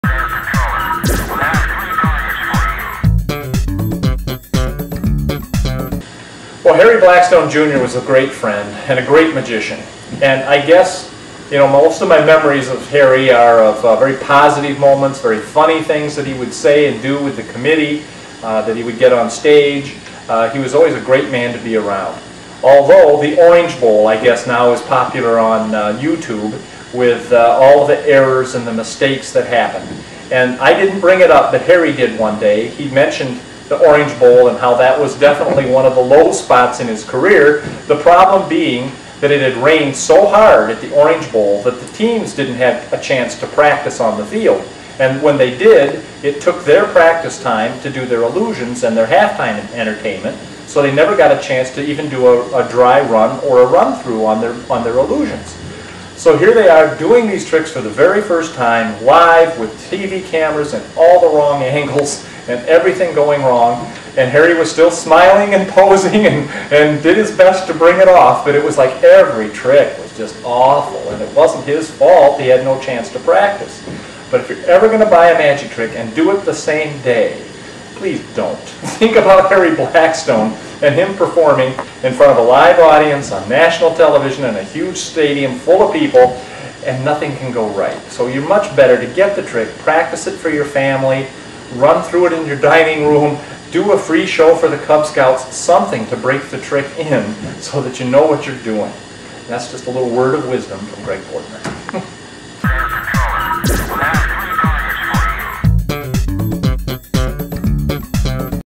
Harry Blackstone Jr
A great supporter of Abbotts Magic, Greg Bordner fondly remembers Harry Blackstone Jr.